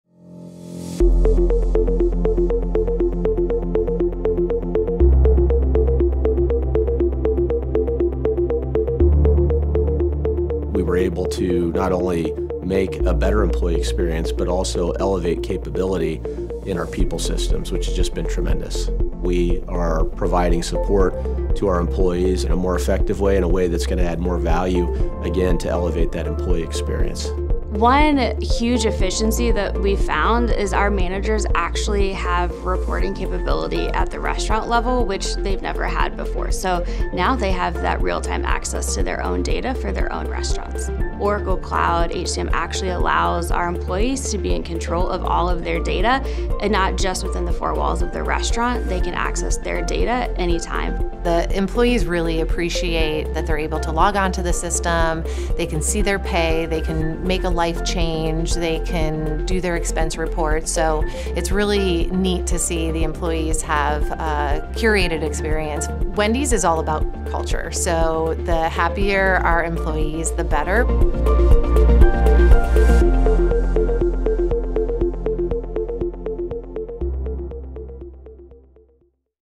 Wendy's: Enhanced Employee Experience with Oracle Cloud HCM Platform –Client Testimonial
Wendy's reveals how KPMG helped them implement the Oracle Cloud HCM system, which gives its employees the ability to manage and control their data anytime and anywhere.  Hear how this advanced system enables employees with real-time data, simplified workflows all while paving the way for continued growth and innovation at Wendy's.